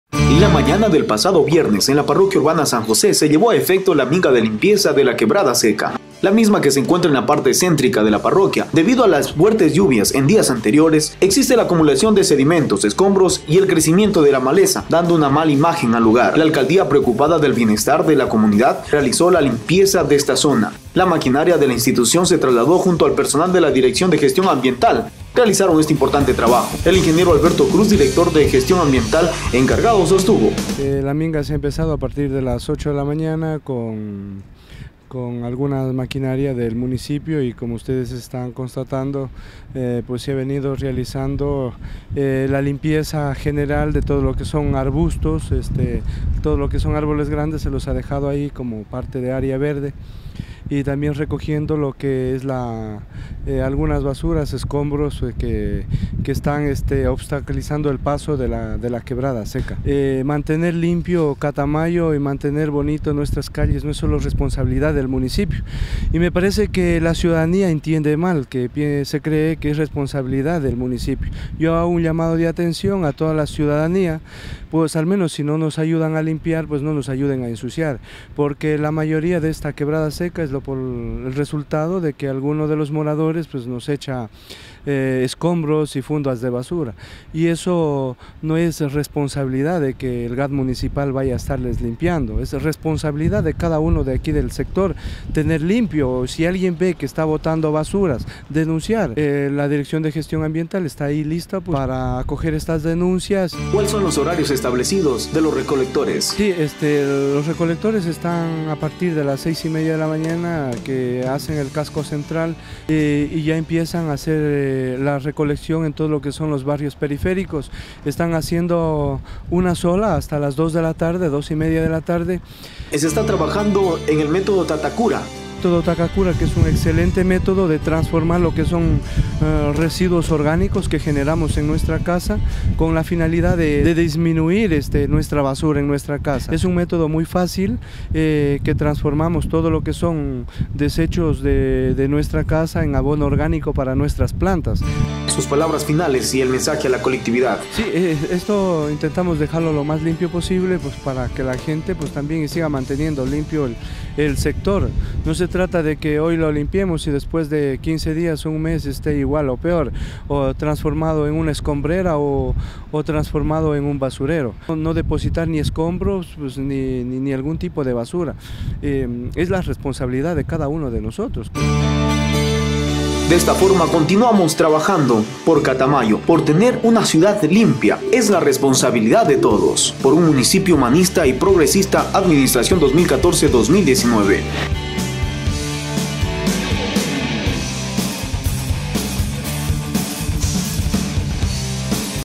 Catamayo.-│Minga de Limpieza de la quebrada seca de San José.│[]
La mañana del pasado viernes, en la parroquia Urbana San José se llevó a efecto,  La minga de limpieza de la quebrada seca, la misma que se encuentra en la parte céntrica de la parroquia, debido a las fuertes lluvias en días anteriores, existe la acumulación de  sedimentos escombros y el crecimiento de maleza  dando una  mala imagen al lugar.

La alcaldía preocupada del bienestar de la comunidad realizó la  limpieza de esta zona,  la maquinaría de la institución se traslado junto al  personal de la  Dirección de Gestión Ambiental, realizaron este importante trabajo.

El Ing. Alberto Cruz, Director de Gestión Ambiental (e) sostuvo, invito a la comunidad que nos ayuden a mantener limpia la ciudad, todos somos responsables de dar ejemplo a los demás, a su vez hago el llamado a la ciudadanía a cuidar este lugar.
 En  próximos dias se implementará el metodo Takakura
en que consiste, utiliza los residuos orgánicos  para sacar el compost.